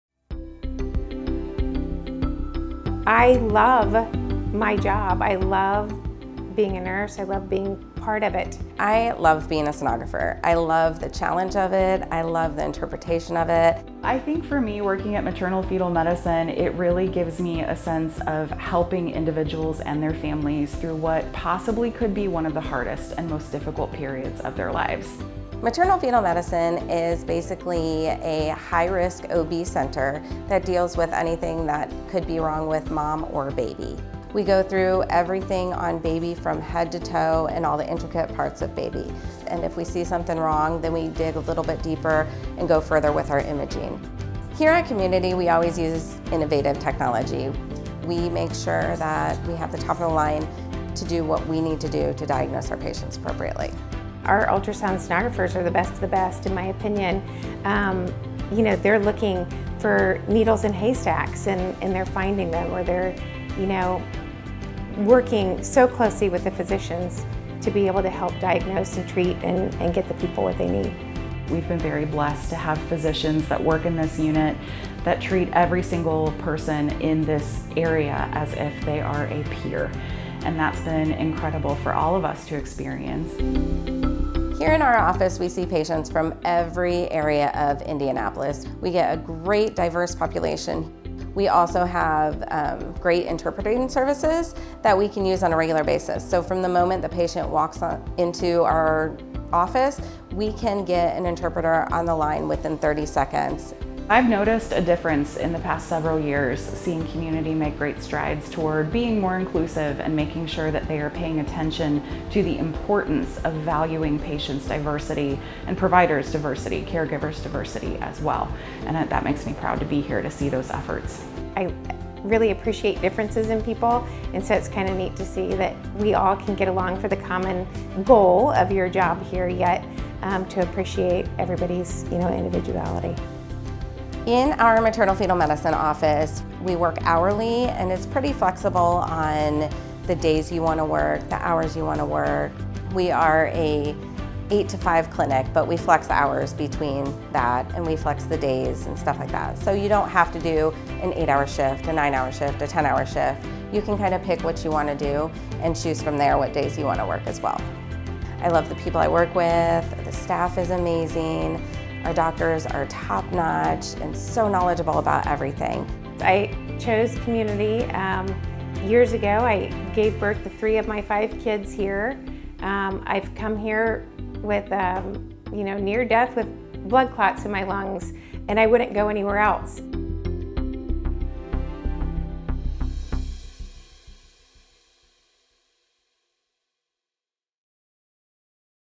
Maternal-Fetal Ultrasound Careers - Community Health Network [LONG]
Explore careers in maternal-fetal ultrasound at Community Health Network. Ultrasound technologists, or sonographers, are trained in imaging procedures that use high frequency sound waves to create pictures of what's going on inside the body. Maternal-fetal medicine ultrasound techs specialize in high-risk pregnancy imaging. Find Community jobs for ultrasound techs and apply today!